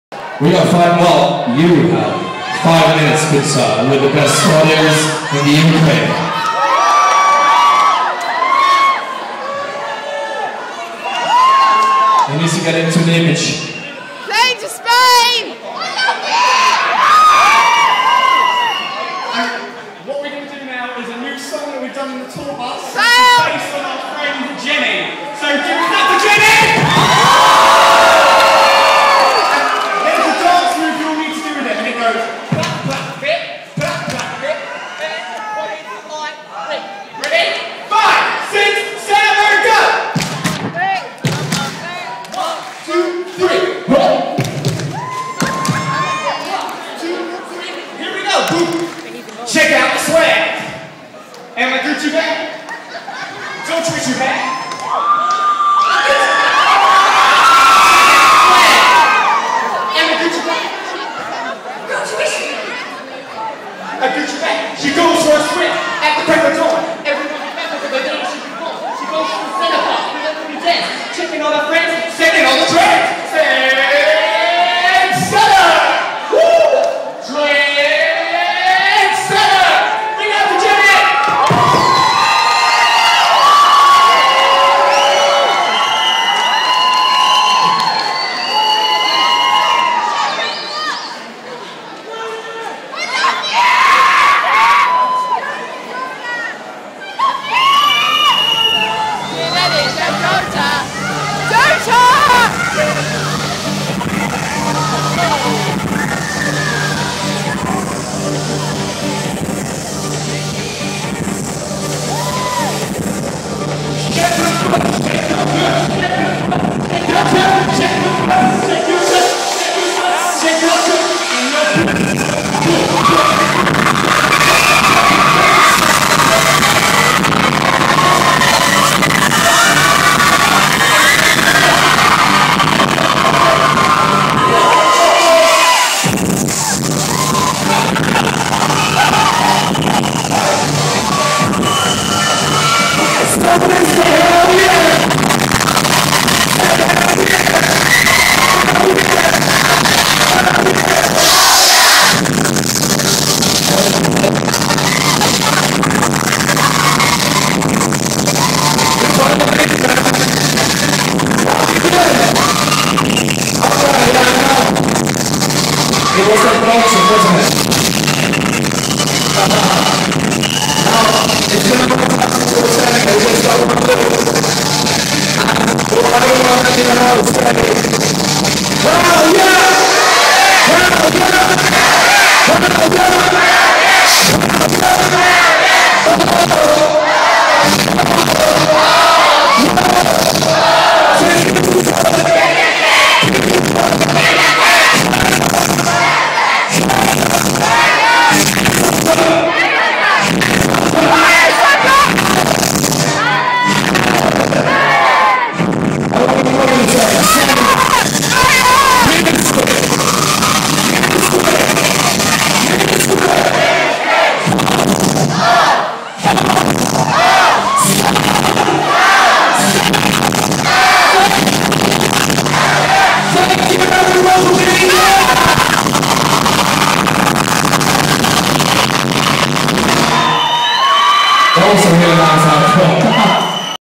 Dance Nation Live 2010: London Finale | Jonas takes to the decks :).
Pretty sweet end to the tour :).

/Smalley ;)